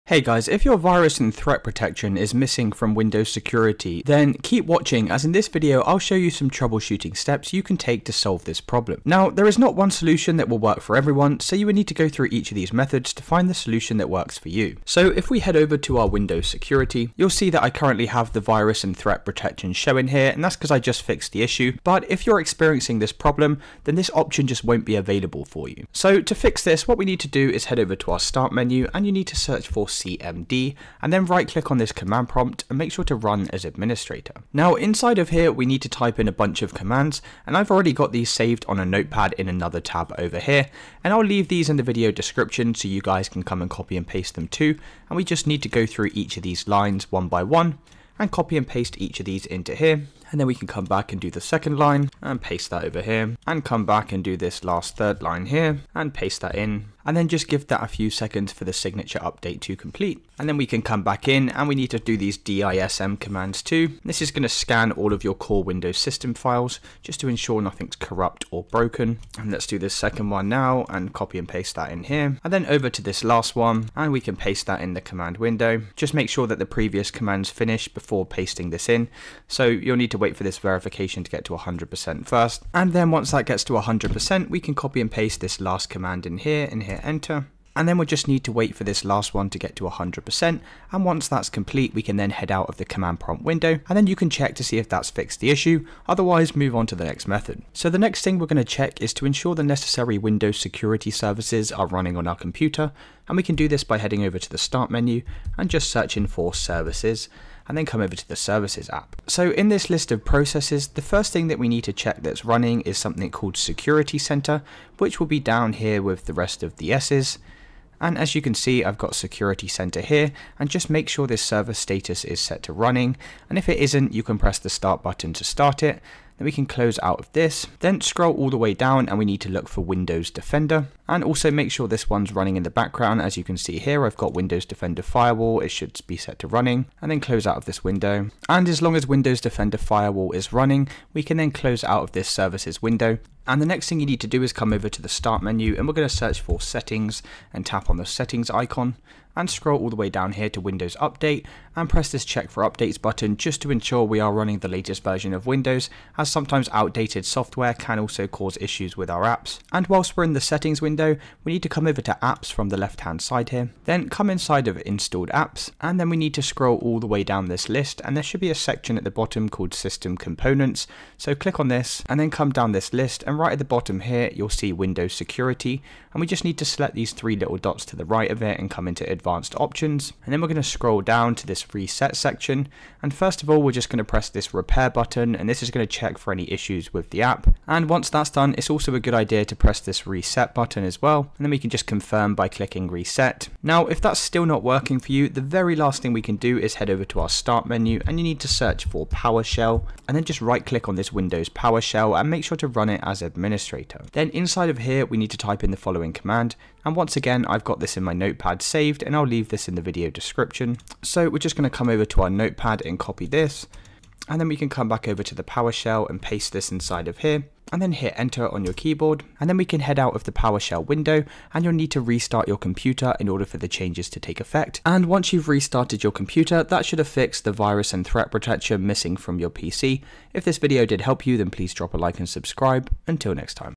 FIX Virus & Threat Protection Missing From Windows Security
In this video, I will guide you through the steps to restore the missing Virus & Threat Protection feature in Windows Security. Whether you're a tech novice or an experienced user, this video provides a clear, easy-to-follow solution to get your Windows Security back to full functionality.

Commands:
cd %ProgramFiles%\Windows Defender
MpCmdRun.exe -removedefinitions -dynamicsignatures
MpCmdRun.exe -SignatureUpdate

Powershell:
Set-MpPreference -UILockdown $false